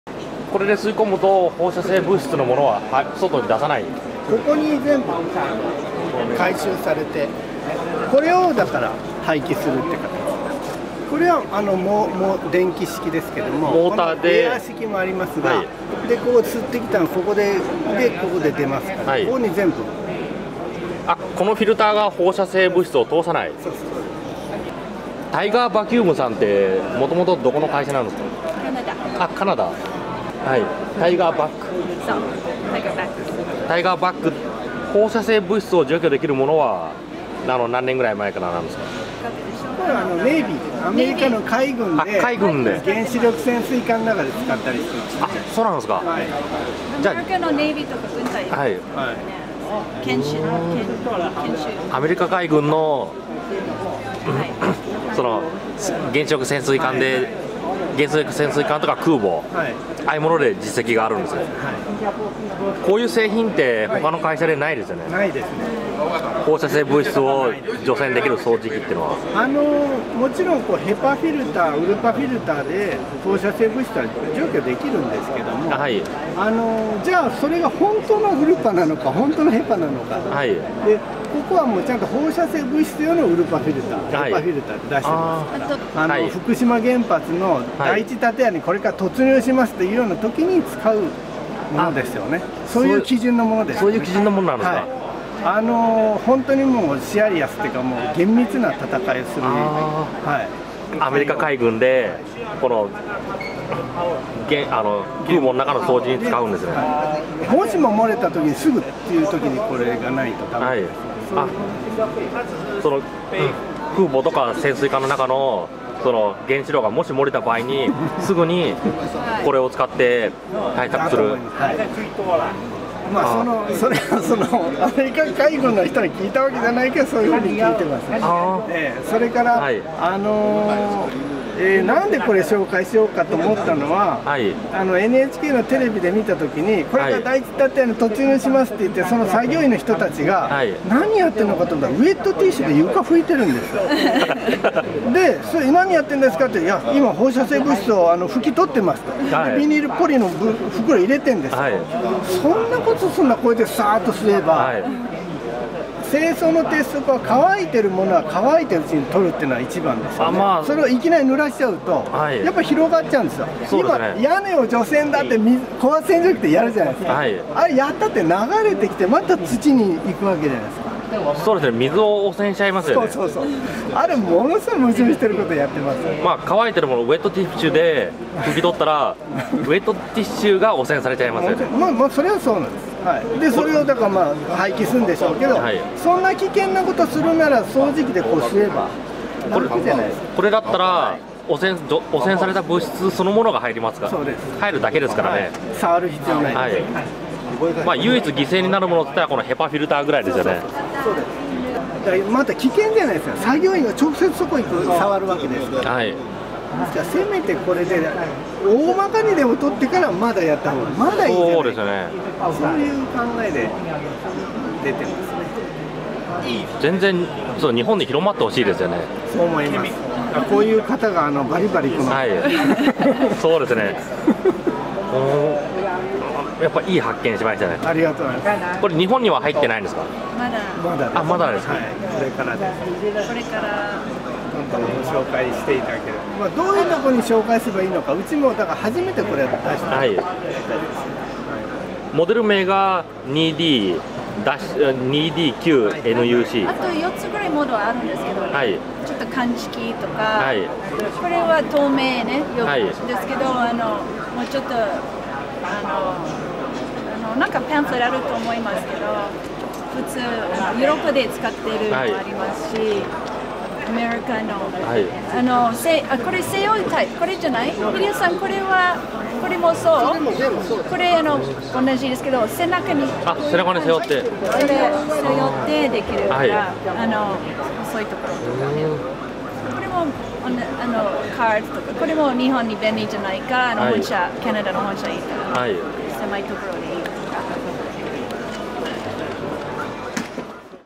放射性物質対応掃除機 加Tiger-Vac社製
日本では放射性物質の除去に『ウェットティッシュ』、『水』などの液体を使用しています。　しかし、「乾燥した汚れは乾燥した状態で除去」するのが効率がよく、放射性物質も例外では有りません。

カナダのTiger-Vac社の『放射性物質・バイオハザード対応掃除機』であれば内蔵『HEPA』,『ULPA』フィルタで放射性物質を吸い込むことができます。　アメリカ海軍の原子力潜水艦で採用実績があると言われています。

水やウェットティッシュによる放射性物質の除去は多くの危険性があります。

・ウェットティッシュが汚染される
・媒体として使用した水が汚染される

と他にも汚染を広げてしまいます。　また、人の手でウェットティッシュや雑巾を使うのは危険が高いです。　そのため、掃除機でできるだけ放射性物質を吸い込んでおけば水を使うより安全です。　本クリーナーで汚染されるものはフィルタのみです。

乾燥した汚れは乾燥したまま処理するのが理想です。　この掃除機は理想に近づけると考えます。

日本総代理店：株式会社フォンシュレーダージャパン ロティナ・ジャパン　部門